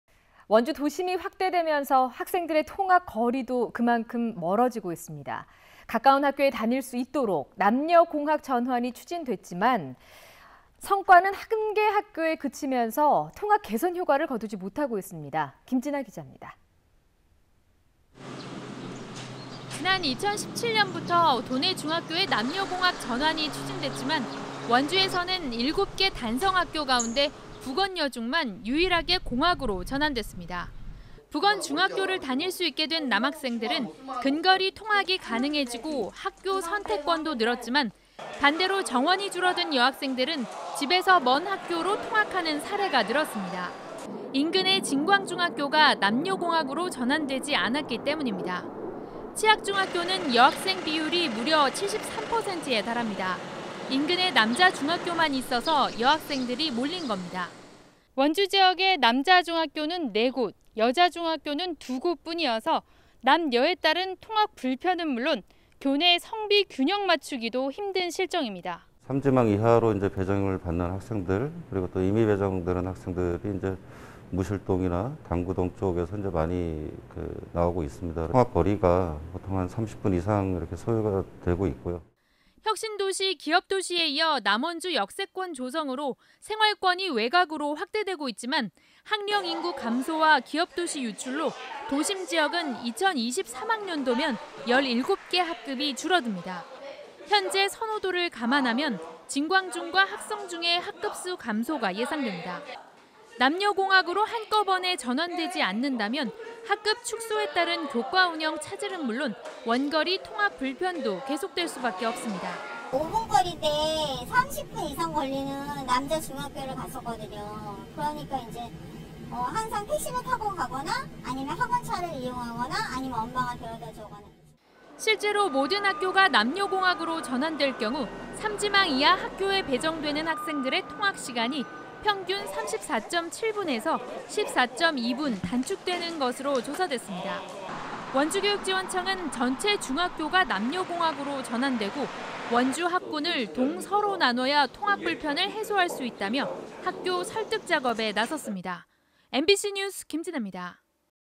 2020. 6. 15 [원주MBC] 남녀공학 전환 미미.. 통학 개선 안 돼
■ 원주 도심이 확대되면서
학생들의 통학 거리도 그만큼 멀어졌죠.

가까운 학교에 다닐 수 있도록
남녀공학 전환이 추진됐지만,

성과는 1개 학교에 그치면서
통학 개선 효과를 거두지 못하고 있습니다.